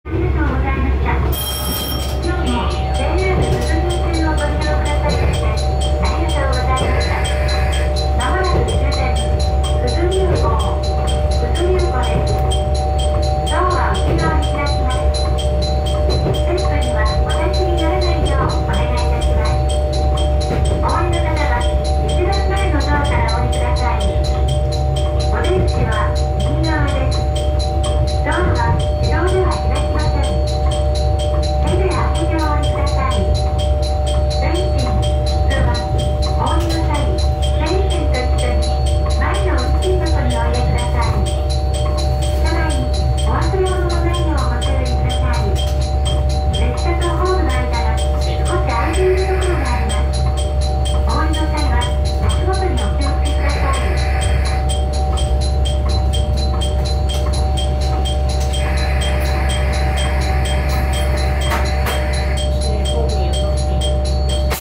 越美北線九頭竜湖駅に到着
生憎の雨の中、越美北線の終点、九頭竜湖駅に到着。乗り鉄としては到着してもすることはなく、同じ列車で福井駅まで戻りました。何が楽しいんだか。